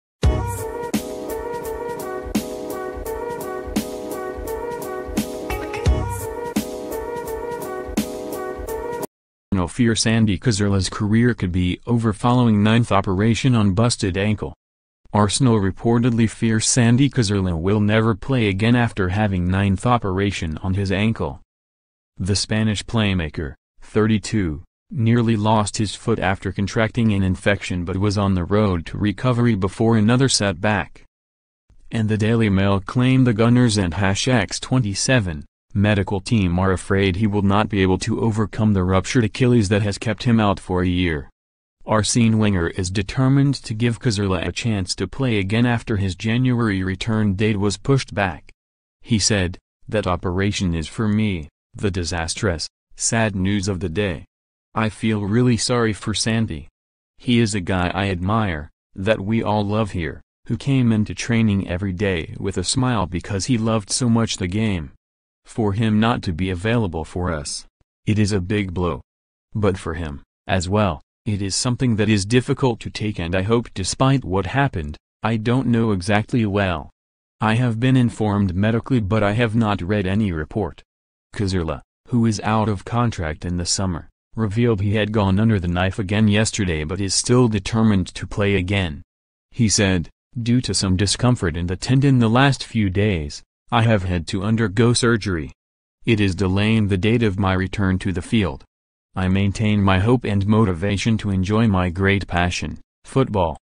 Arsenal fear Santi Cazorla’s career could be over following ninth operation on busted ankle
ARSENAL reportedly fear Santi Cazorla will never play again after having  NINTH operation on his ankle.

The Spanish playmaker, 32, nearly lost his foot after contracting an infection but was on the road to recovery before another set-back.

And the Daily Mail claim the Gunners&x27; medical team are afraid he will not be able to overcome the ruptured Achilles that has kept him out for a year.

Arsene Wenger is determined to give Cazorla a chance to play again after his January return date was pushed back.